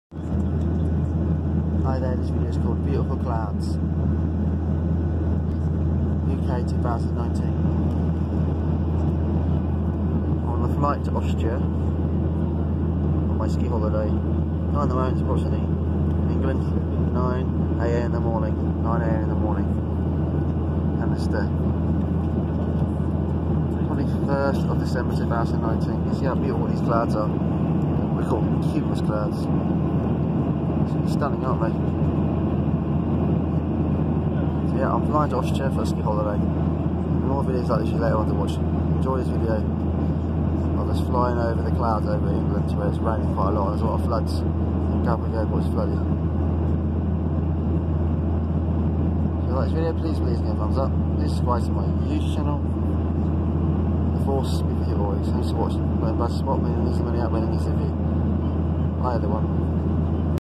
BEAUTIFUL CLOUDS UK 2019.
TALK ABOUT THE BEAUTIFUL CLOUDS IN THE BACKGROUND AND THE THE FLOODS ACROSS THE SOUTH OF EBGLAND.